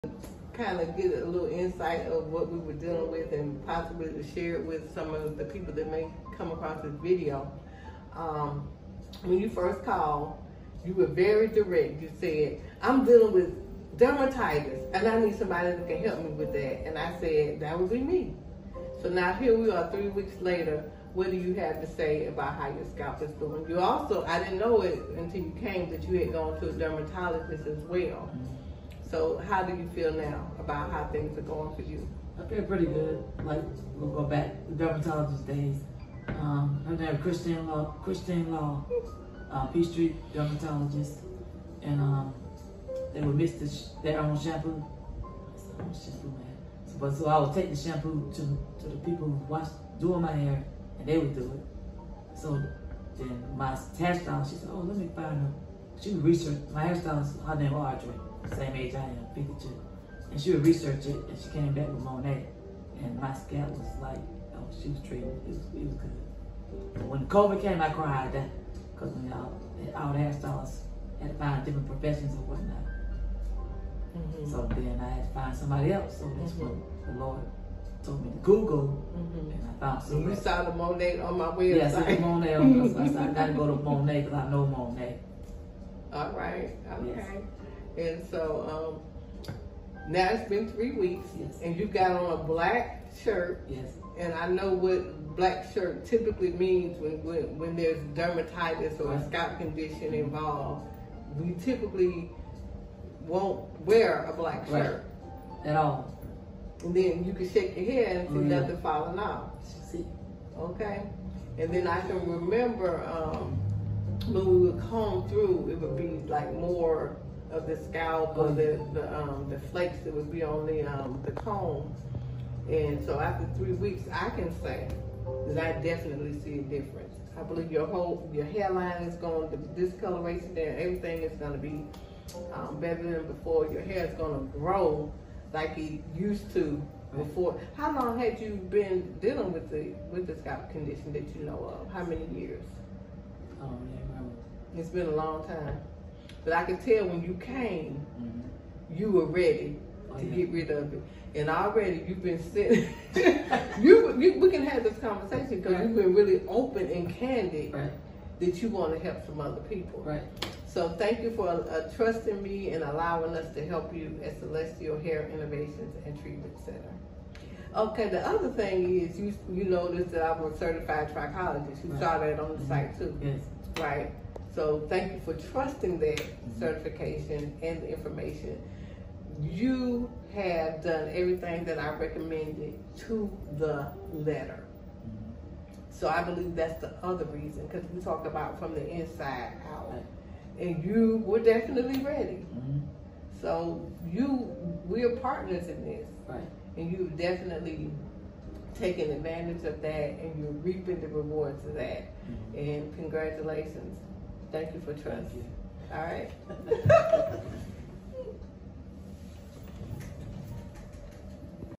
Dealing with dermatitis one on one😇
Live conversation about a serious scalp situation!